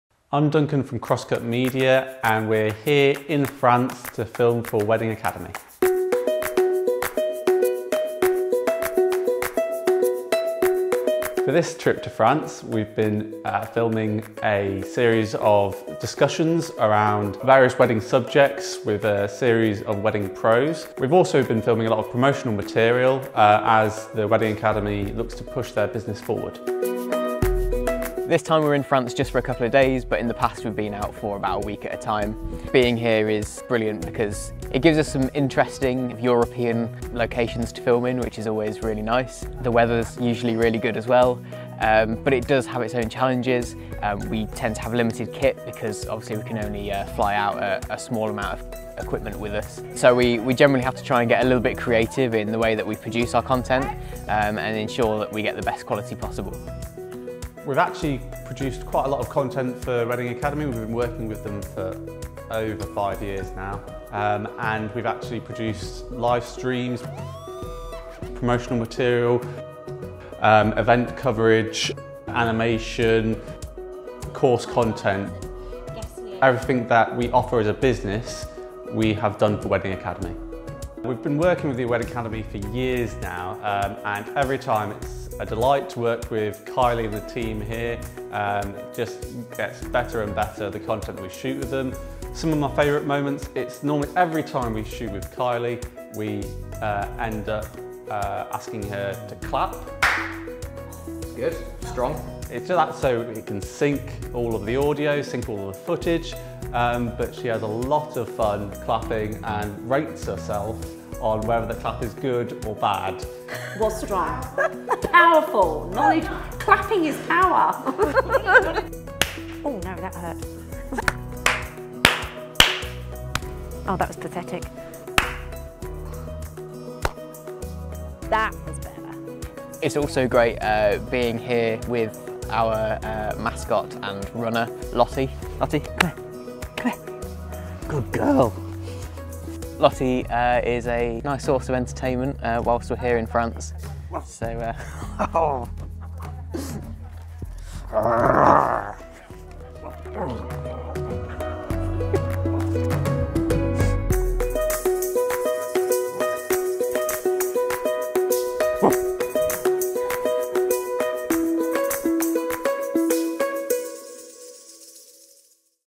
Behind the Scenes - Wedding Academy Global
Take a look behind the scenes on one of our international shoots with Wedding Academy Global